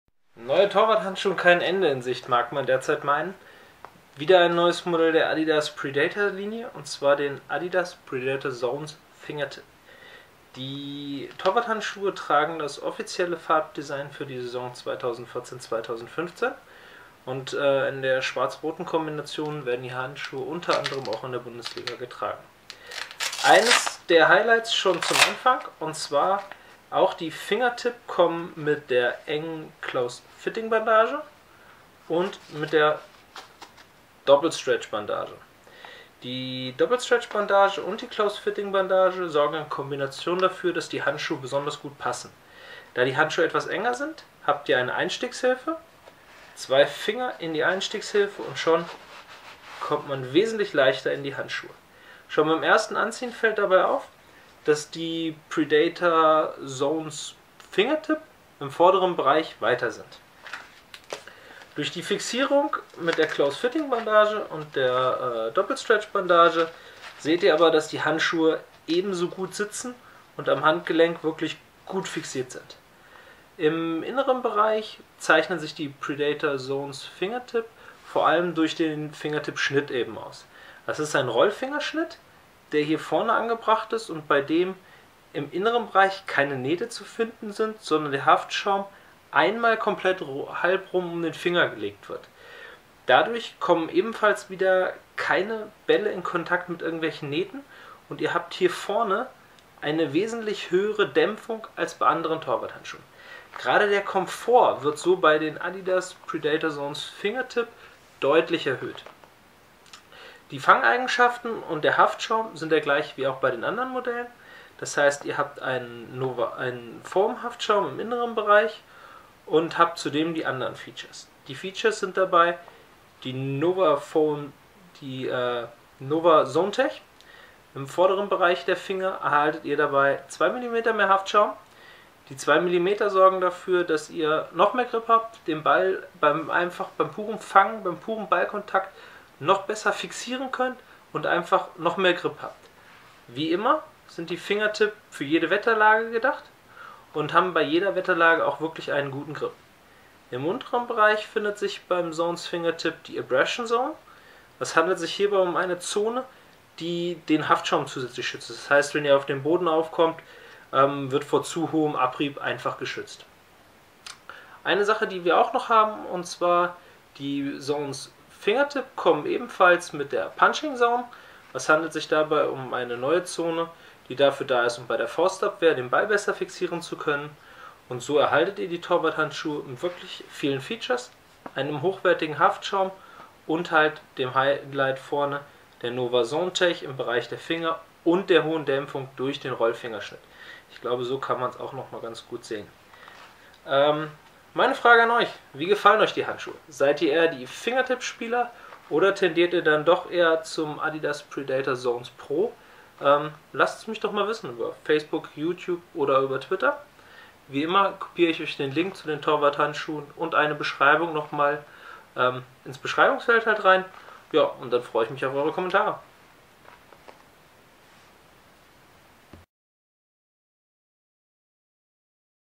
Adidas Predator Zones Fingertip 2014/2015 - Torwarthandschuhe
Die Adidas Predator Zones Fingertip gibt es als Torwarthandschuhe im speziellem Rund-Finger zu kaufen. Anderes als ale anderen Torwarthandschuhe von Adidas verfügen die Predator Zones Fingertip damit über eine komplett eigene Schnittform.

Durch den so bezeichneten Rolled-Cut (Rollfinger) wird der Haftschaum um die Finger gedreht. Die Adidas Predator Zones Fingertip 2014/2015 erhalten mit dieser Schnittform eine noch bessere Dämpfung. Hinzu kommt das die Adidas Predator Zones Fingertip Torwarthandschuhe so eine Schnittform haben die verhindert das einzelne Nähte mit dem Ball in Kontakt kommen könnten.

Neben der eigenen Schnittform setzen die Adidas Predator Zones Fingertip Torwarthandschuhe auf Features die auch bei anderen Handschuhen der Saison 2014/2015 verwendet werden. Neben der Nova Zone Tech (2mm mehr Haftschaum) im Bereich der Finger kommt auch die Punching Zone und die Abrasion Zone zum Einsatz.

Die Feature Übersicht der Adidas Predator Zones Fingertip 2014/2015

- Punching Zone (Faustabwehr)
- Abrasion Zone (Haftschaum Schutz)
- Nova Foam Zone (mehr Grip)
- Close Fitting Bandage
- Doppel Stretch Bandage
- Rolled-Cut

Das Design und die Farben der Adidas Predator Zones Fingertip 2014/2015 wird zudem auch im Profi Fußball getragen. Torhüter die Manuel Neuer oder Rene Adler setzen auf den Look der Adidas Predator Zones Fingertip in der Saison 2014/2015.